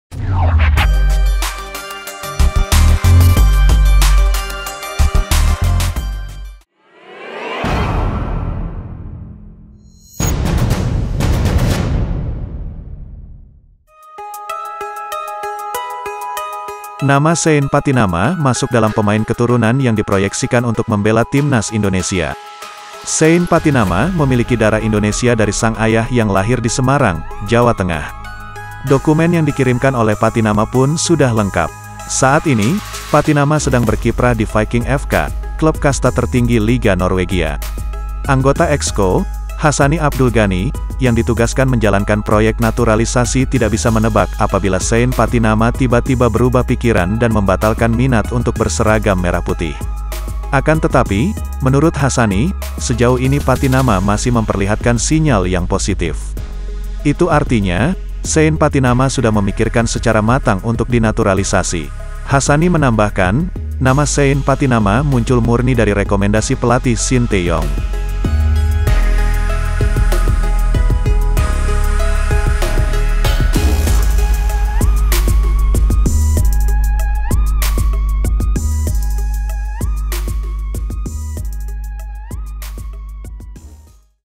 Shayne Pattynama Pemain Keturunan yang Direkomendasikan Shin Tae-yong
Nama Shayne Pattynama masuk dalam pemain keturunan yang diproyeksikan untuk membela timnas Indonesia.

Like & Follow Facebook:
BolaSport.com